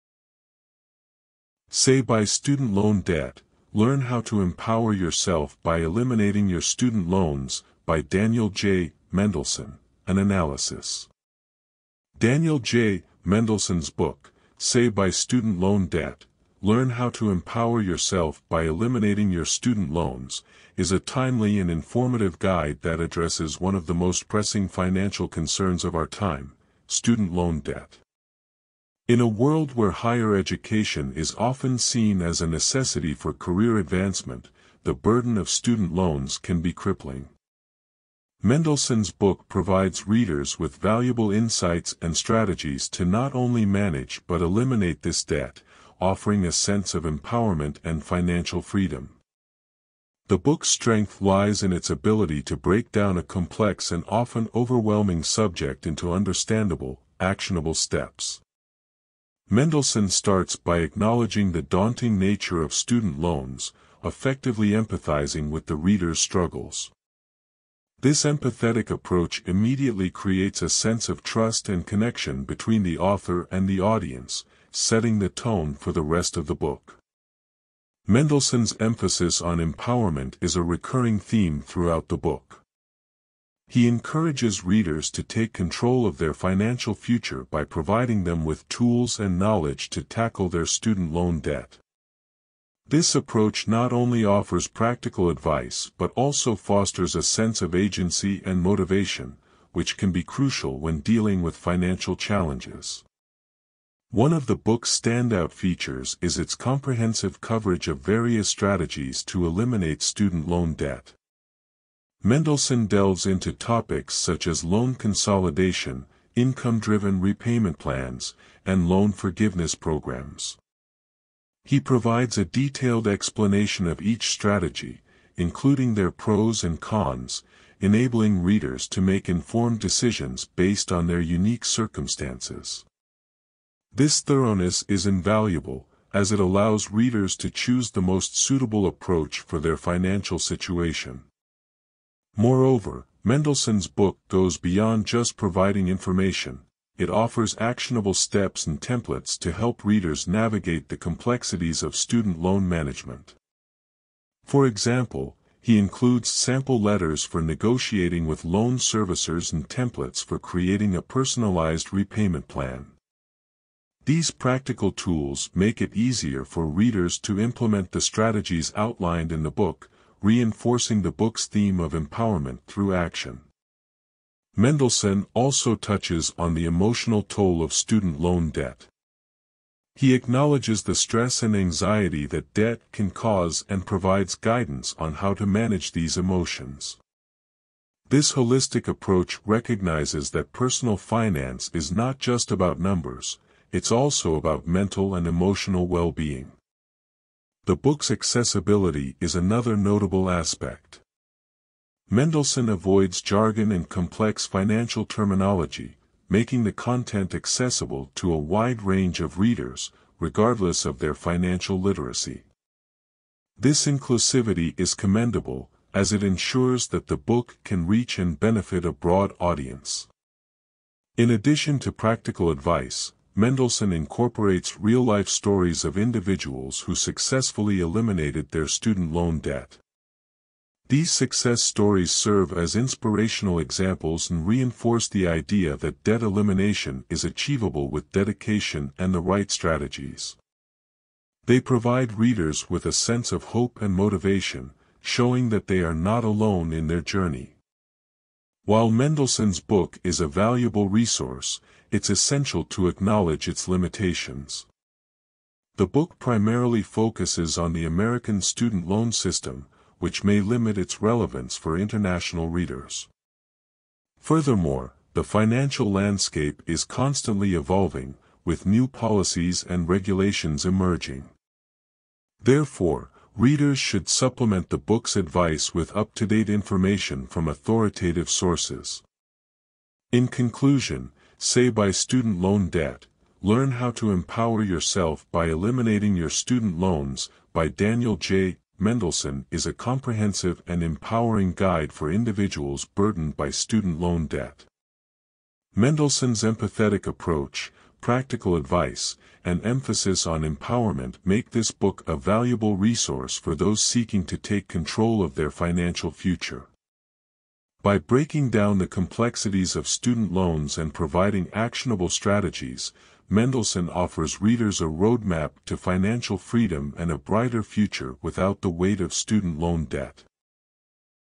"Say BYE Student Loan Debt" By Daniel J. Mendelson
"Say BYE Student Loan Debt: Learn How to Empower Yourself by Eliminating Your Student Loans" by Daniel J. Mendelson: An AnalysisDaniel J. Mendelson's book, "Say BYE Student Loan Debt: Learn How to Empower Yourself by Eliminating Your Student Loans," is a timely and informative guide that addresses one of the most pressing financial concerns of our time - student loan debt. In a world where higher education is often seen as a necessity for career advancement, the burden of student loans can be crippling. Mendelson's book provides readers with valuable insights and strategies to not only manage but eliminate this debt, offering a sense of empowerment and financial freedom.The book's strength lies in its ability to break down a complex and often overwhelming subject into understandable, actionable steps. Mendelson starts by acknowledging the daunting nature of student loans, effectively empathizing with the reader's struggles. This empathetic approach immediately creates a sense of trust and connection between the author and the audience, setting the tone for the rest of the book.Mendelson's emphasis on empowerment is a recurring theme throughout the book. He encourages readers to take control of their financial future by providing them with tools and knowledge to tackle their student loan debt. This approach not only offers practical advice but also fosters a sense of agency and motivation, which can be crucial when dealing with financial challenges.One of the book's standout features is its comprehensive coverage of various strategies to eliminate student loan debt. Mendelson delves into topics such as loan consolidation, income-driven repayment plans, and loan forgiveness programs. He provides a detailed explanation of each strategy, including their pros and cons, enabling readers to make informed decisions based on their unique circumstances. This thoroughness is invaluable, as it allows readers to choose the most suitable approach for their financial situation.Moreover, Mendelson's book goes beyond just providing information; it offers actionable steps and templates to help readers navigate the complexities of student loan management. For example, he includes sample letters for negotiating with loan servicers and templates for creating a personalized repayment plan. These practical tools make it easier for readers to implement the strategies outlined in the book, reinforcing the book's theme of empowerment through action.Mendelson also touches on the emotional toll of student loan debt. He acknowledges the stress and anxiety that debt can cause and provides guidance on how to manage these emotions. This holistic approach recognizes that personal finance is not just about numbers; it's also about mental and emotional well-being.The book's accessibility is another notable aspect. Mendelson avoids jargon and complex financial terminology, making the content accessible to a wide range of readers, regardless of their financial literacy. This inclusivity is commendable, as it ensures that the book can reach and benefit a broad audience.In addition to practical advice, Mendelson incorporates real-life stories of individuals who successfully eliminated their student loan debt. These success stories serve as inspirational examples and reinforce the idea that debt elimination is achievable with dedication and the right strategies. They provide readers with a sense of hope and motivation, showing that they are not alone in their journey.While Mendelson's book is a valuable resource, it's essential to acknowledge its limitations. The book primarily focuses on the American student loan system, which may limit its relevance for international readers. Furthermore, the financial landscape is constantly evolving, with new policies and regulations emerging. Therefore, readers should supplement the book's advice with up-to-date information from authoritative sources.In conclusion, "Say BYE Student Loan Debt: Learn How to Empower Yourself by Eliminating Your Student Loans" by Daniel J. Mendelson is a comprehensive and empowering guide for individuals burdened by student loan debt. Mendelson's empathetic approach, practical advice, and emphasis on empowerment make this book a valuable resource for those seeking to take control of their financial future. By breaking down the complexities of student loans and providing actionable strategies, Mendelson offers readers a roadmap to financial freedom and a brighter future without the weight of student loan debt.